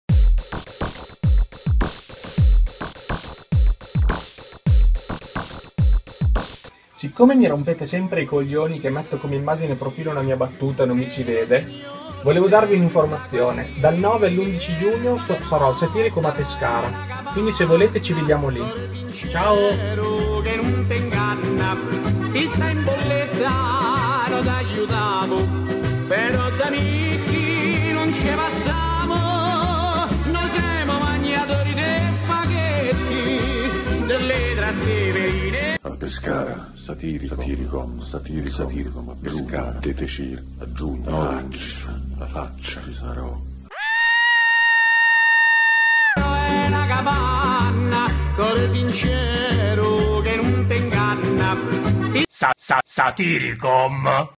costole #4 | SATYRICOM | Rassegna Nazionale di Satira Web
SATYRICOM presenta Costole!
Fino a Giugno, doppio appuntamento, ogni Martedì e Giovedì con un nuovo video.
Non è andato a Stoccolma per il Nobel, ma sarà a Pescara per essere adorato dalle folle festanti, il Bob Dylan di Piacenza, alias Daniele Villa, alias Zziagenio78. E visto che le folle restanti stanno là, ci mettiamo pure il fascino indo-laziale del nostro unico e solo guru personale Le più belle frasi di Osho al secolo Federico Palmaroli.
Ancora non riuscite ad essere folla festante al 100%?
E allora sconvolgetevi con l'ars esoterica della matita più accattivante della costa occidentale, quella di Fabio Magnasciutti.
Pescara 9, 10 e 11 Giugno 2017
Torna SATYRICOM, Rassegna Nazionale di Satira Web.
72 ore di spettacoli, incontri, scontri, workshop, presentazioni, mostri, mostre, performance, dèfaillance e reading dal vivo.